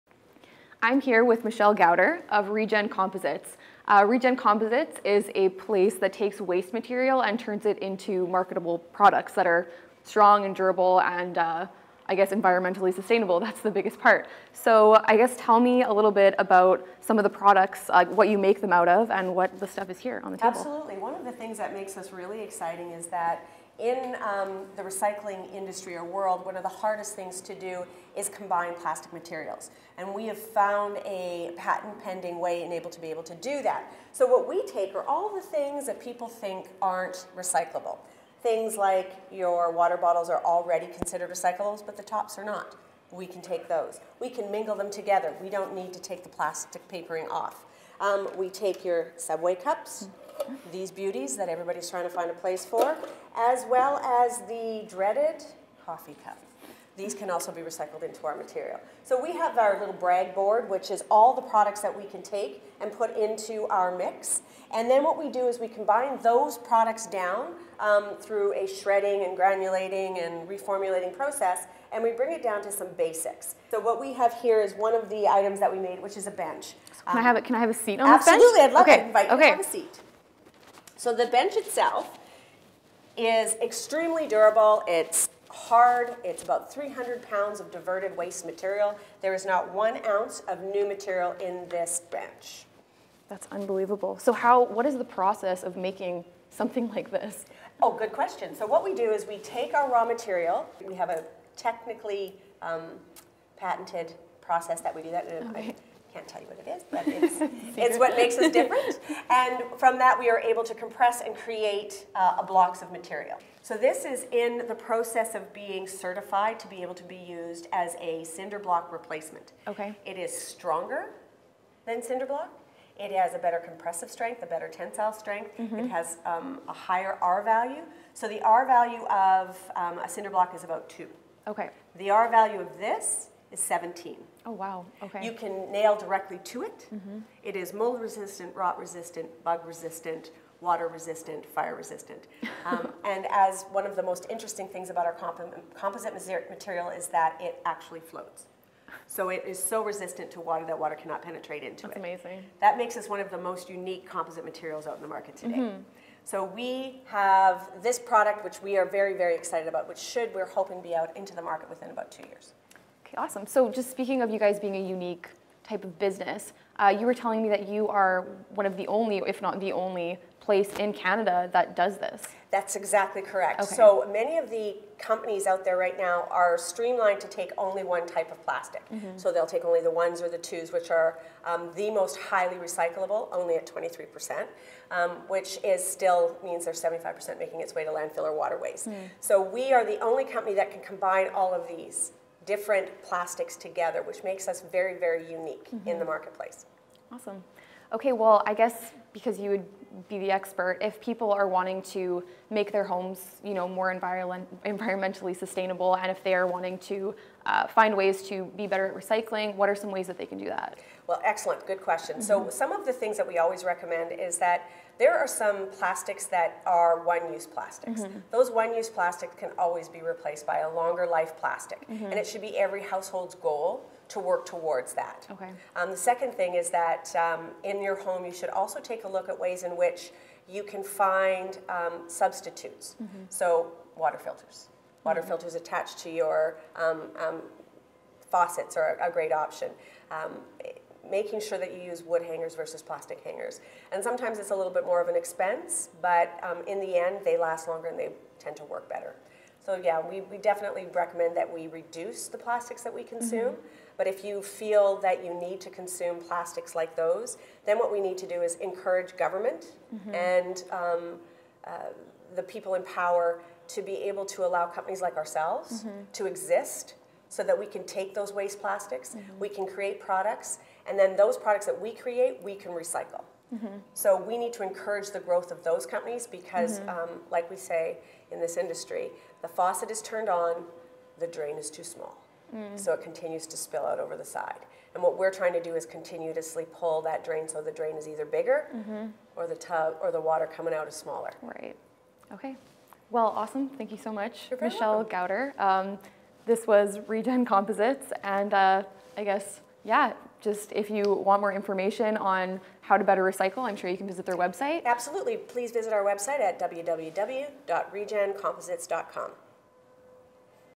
ReGen Composites
ReGen Composites takes waste materials and turns them into marketable products. We spoke with COO Michelle Gowdar of ReGen about some of the products they create.